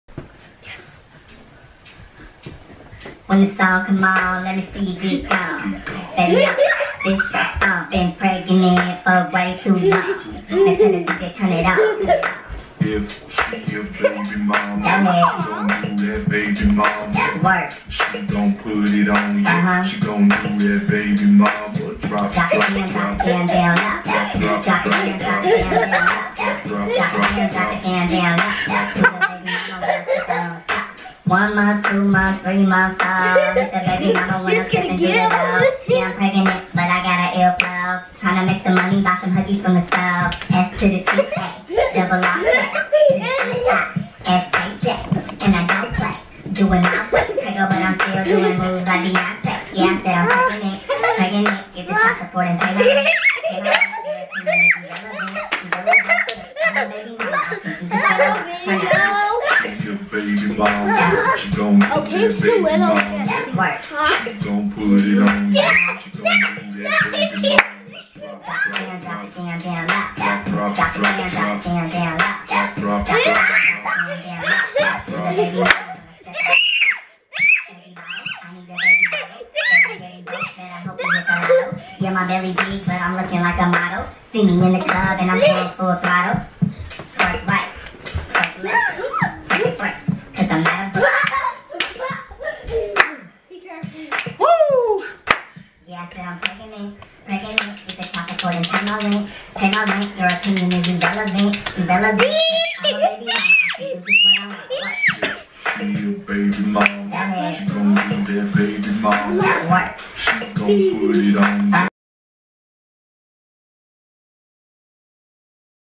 Baby Momma Dance Challenge!! (Hillarious!!) MUST SEE!!!! (38 Weeks Pregnant)
Here it is!! You asked for it!! We delivered!! BABBY MOMMA DANCE CHALLENGE!! MUST SEE!!! Hillarious!!! Lollollol Share please!!